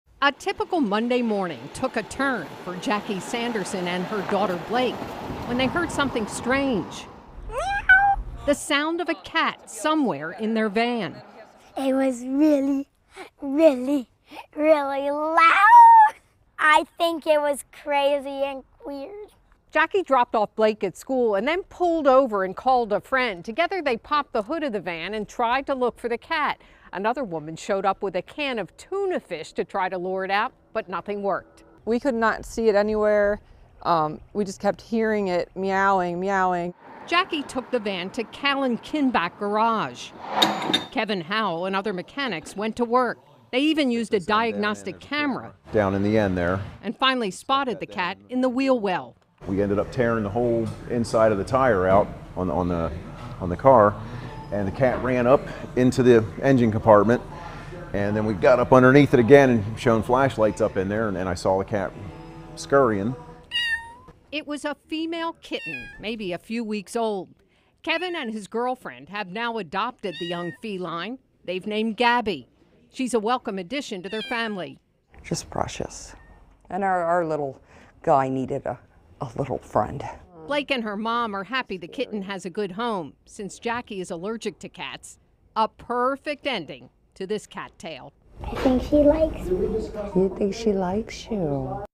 Kitten trapped in minivan's wheel well
A woman in Cumberland County heard meowing coming from somewhere inside her minivan, but she couldn't find the cat.